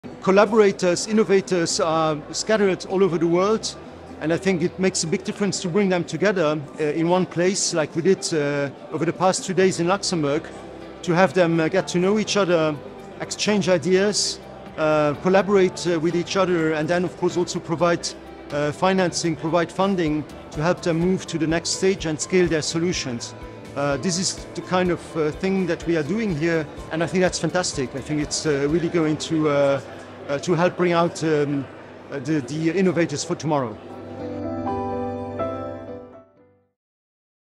We need innovative solutions. | Franz Foyot speaks about the Humanitarian Innovation Accelorator
💡 Franz Foyot, the Luxembourg Minister for Development Cooperation and Humanitarian Affairs speaks about the mission and importance of the Humanitarian Innovation Accelerator Programme and Pitch Event. More info below. ⬇️

🌟 In June 2023, 16 high-impact global innovation teams, armed with tech-driven solutions to address humanitarian challenges, gathered in Luxembourg after completing an intensive online bootcamp run by the UN WFP Innovation Accelerator. The Humanitarian Innovation Accelerator Pitch Event became a captivating platform, showcasing their groundbreaking solutions and fostering dynamic conversations and collaborations among investors, donors, government officials, industry leaders, humanitarian actors, and private sector leaders.

🌟 The Humanitarian Innovation Accelerator Programme, powered by the WFP Innovation Accelerator, the Government of the Grand-Duchy of Luxembourg, and the Austrian Development Cooperation (ADC), aims to support technology-powered solutions addressing global humanitarian issues. Selected ventures and solution providers will receive invaluable financial, technical, and methodological support. Together, let's bring our brightest minds together and transform the lives of vulnerable communities worldwide.